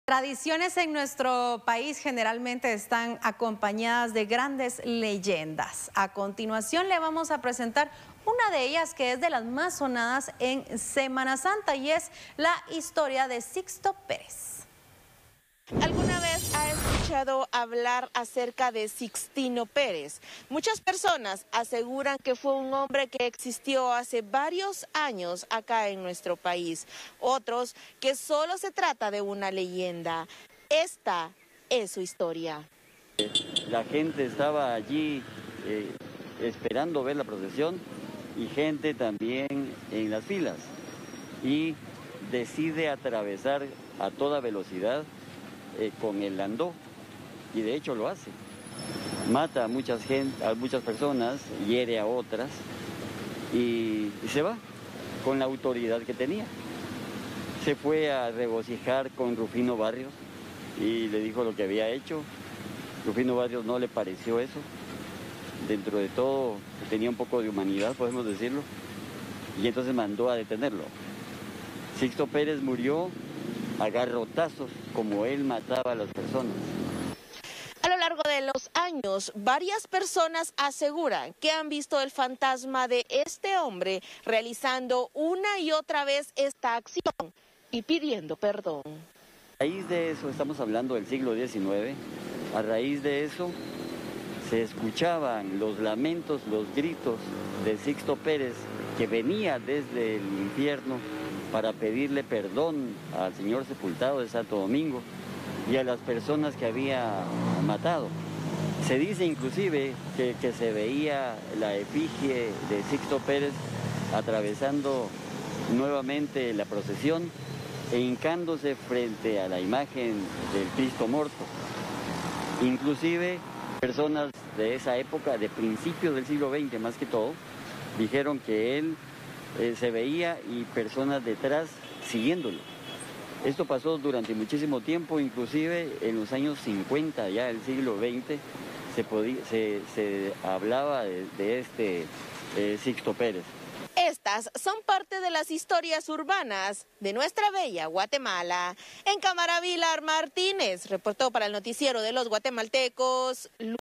Conozca la leyenda de Sixto Pérez
Personas aseguran que han visto el fantasma de este hombre

Nuestra compañera Luisa Velásquez nos cuenta más sobre la leyenda de Sixto Pérez. Visita nuestro sitio